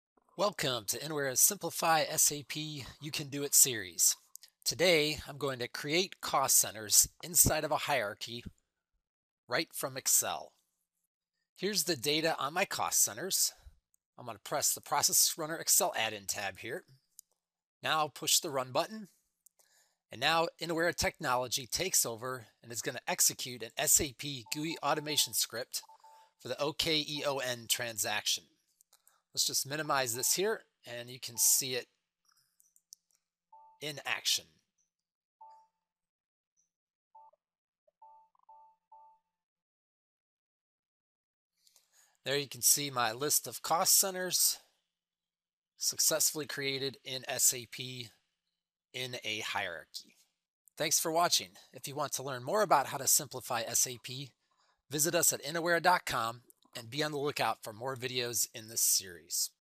OKEON - Change Standard Hierarchy From Excel
In this quick video an end user creates cost centers inside of a hierarchy in SAP by running a GUI automation script for the Tcode OKEON right from Excel.  Utilizing Innowera's Process Runner Excel Add-in technology the end user simply presses a run button in the Excel Add-in, to execute the script.